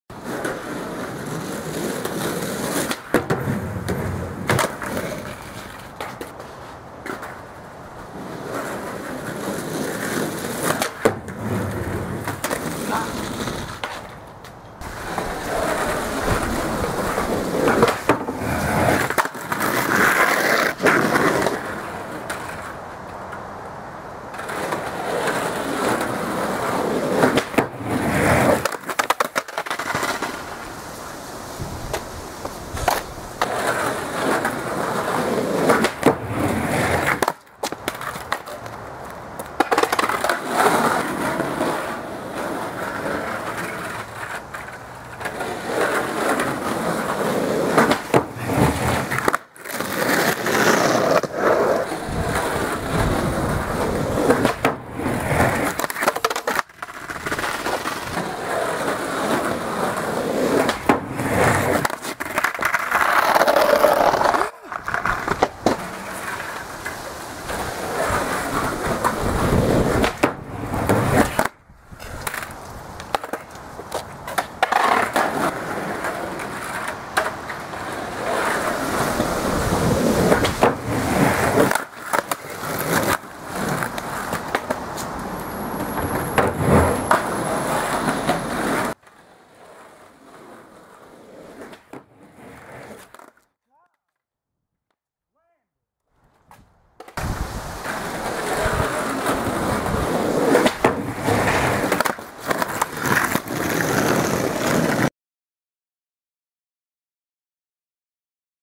Close Enough?
Fs 180 to switch manual. Fs 180 to switch manual bs 180 out.